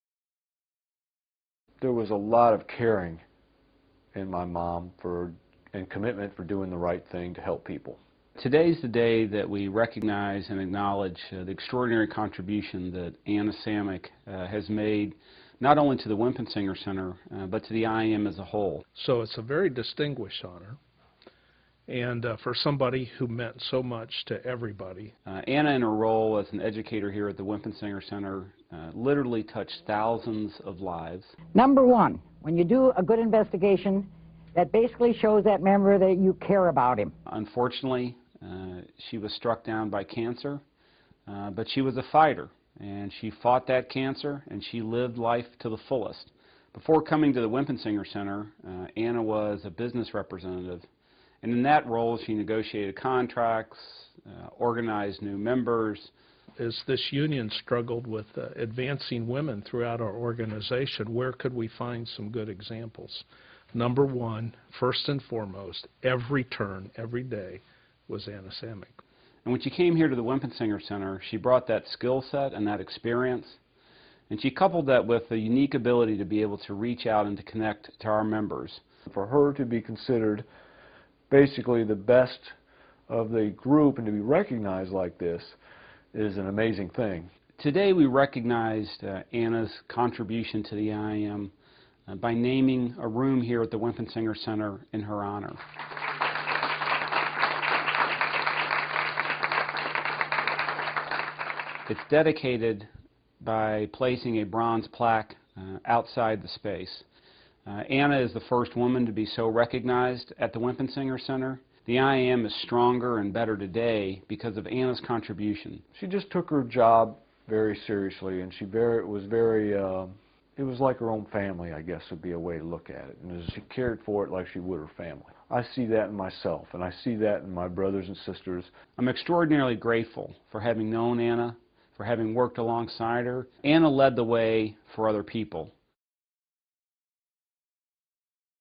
Remembering An IAM Pioneer
October 22, 2009 - Anna J. Samick (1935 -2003) was one of the IAM's first woman business representatives, and the first woman educator at the Winpisinger Center. Saturday, the Machinists Union honored her memory by dedicating a room in her name.